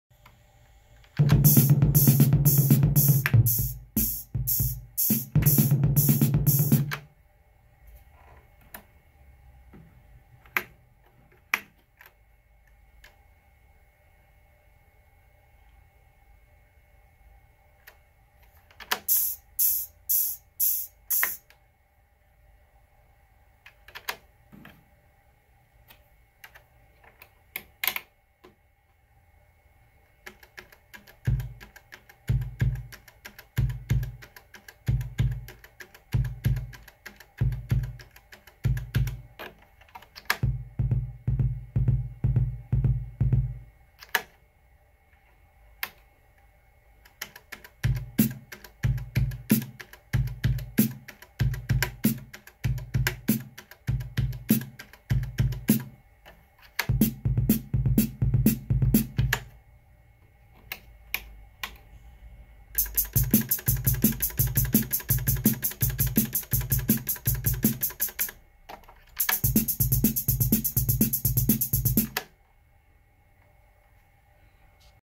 Roland CompuRhythm CR8000 - Demo #1
Programming a pattern. Previously the programming mode did not work.